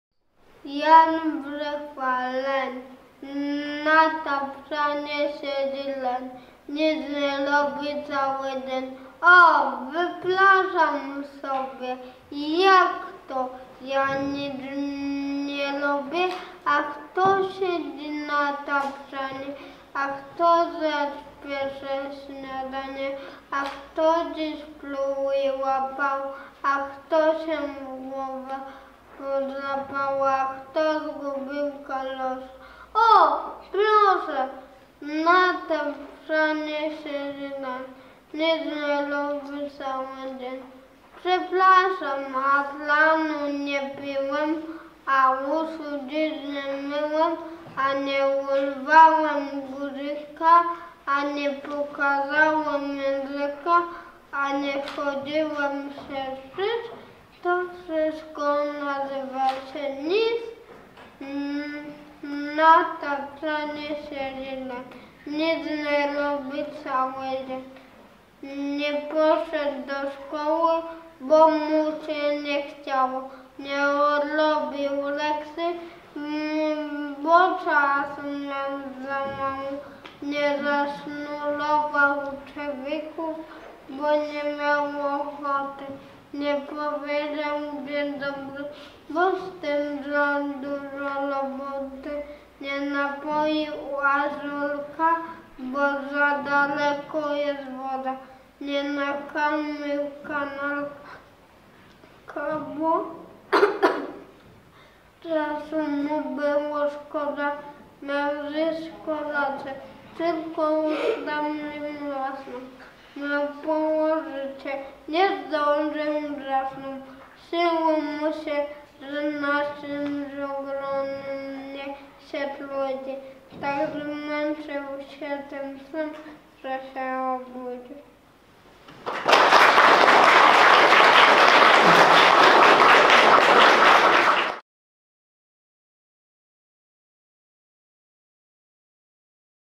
Olaf Ziajor z 3b recytuje wiersz „Leń”
Olaf Ziajor z 3b recytuje wiersz „Leń”. Konkurs recytatorski dla klas IV - VII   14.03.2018 r. Szkoła Podstawowa Nr 4 w Andrychowie